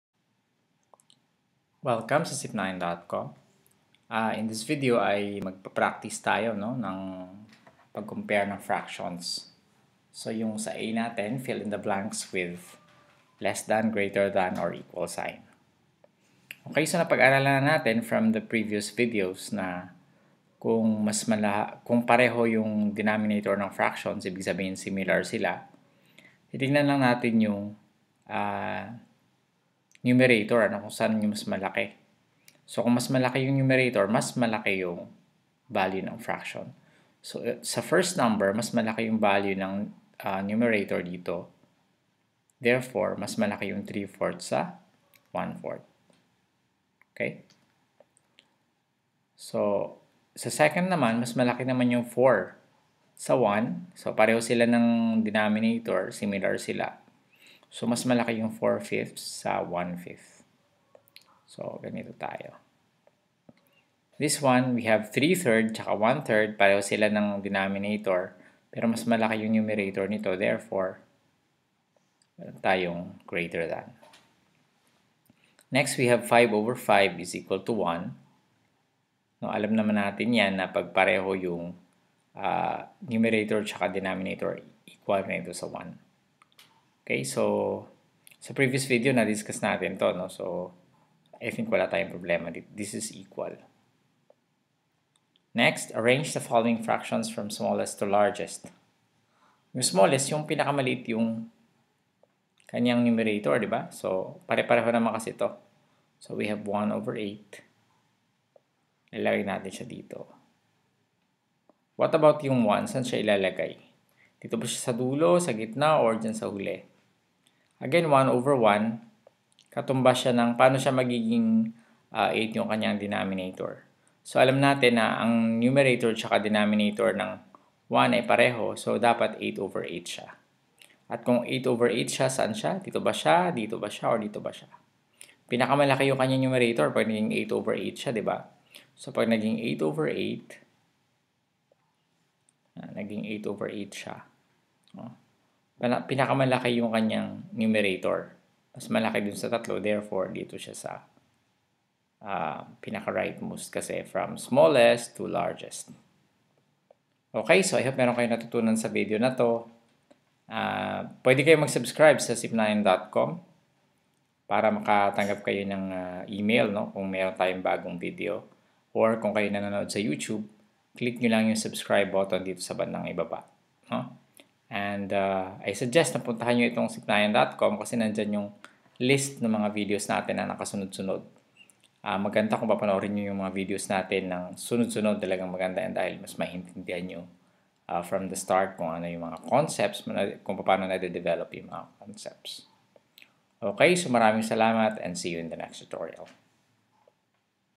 FR05 Comparing Fractions Examples
In this video we are going to learn how to compare fractions.